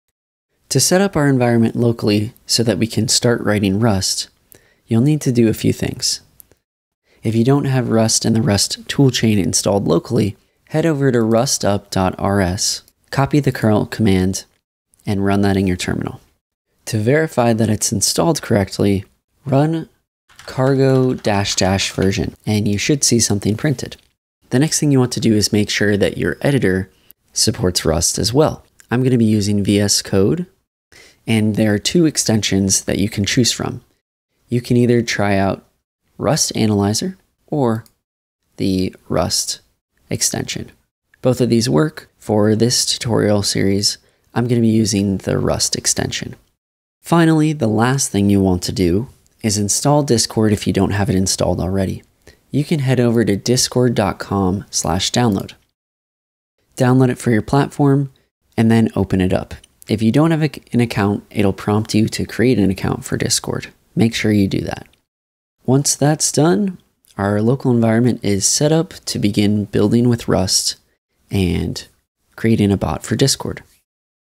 Discord Bot with Rust and Serenity - #1 Set Up Environment and Sign Up for Discord
In this video, Joe will walk you through the prerequisites for following along in this video series. We’ll cover the following:

Social Accounts